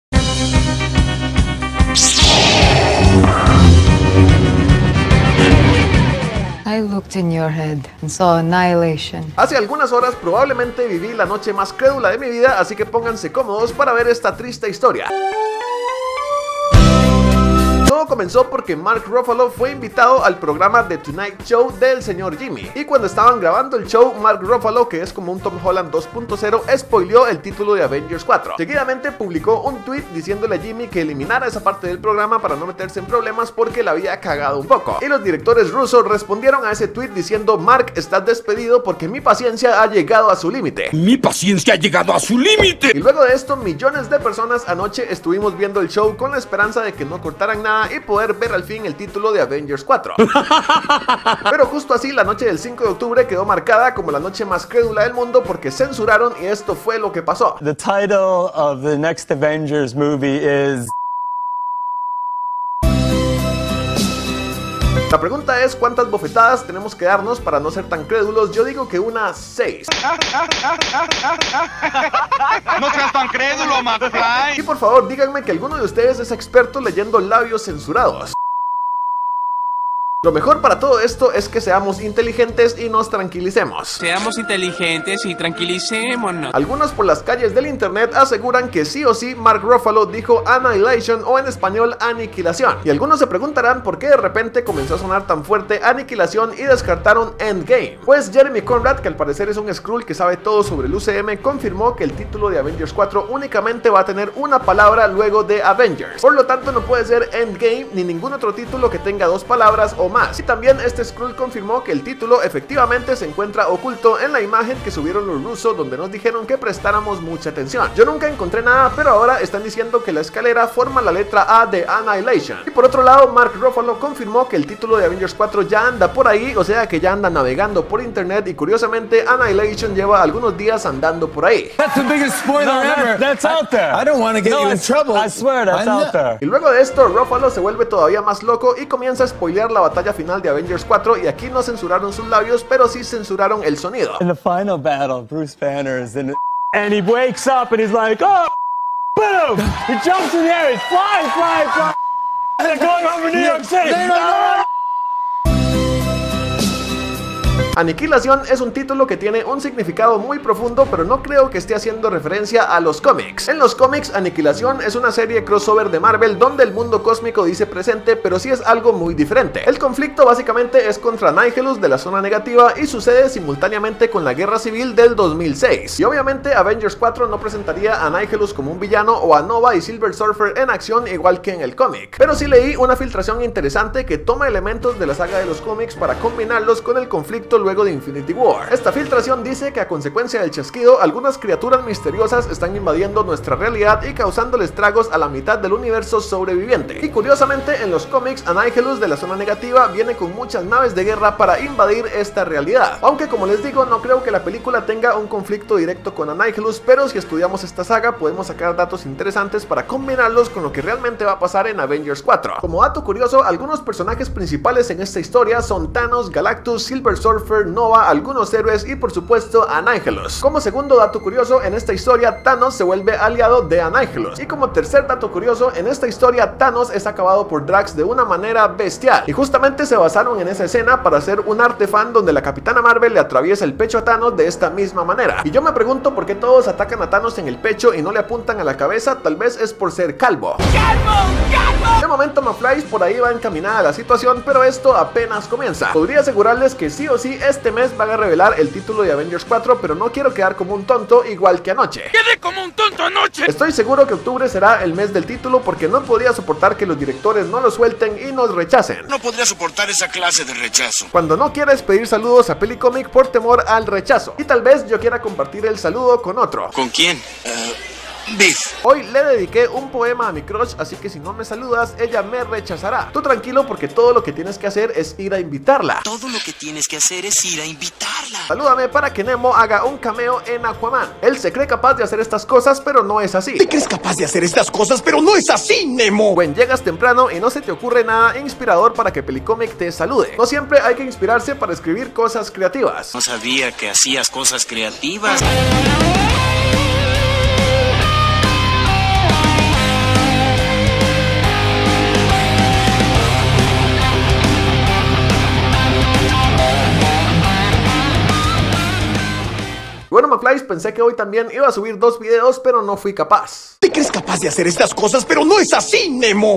IMPERDIBLE título de Avengers 4 y batalla FINAL ¡Annihilation! + spoilers
Mark Ruffalo revela spoilers de Avengers 4, Título Aniquilación y la batalla final contra Thanos + breve análisis a Annihilation en los cómics!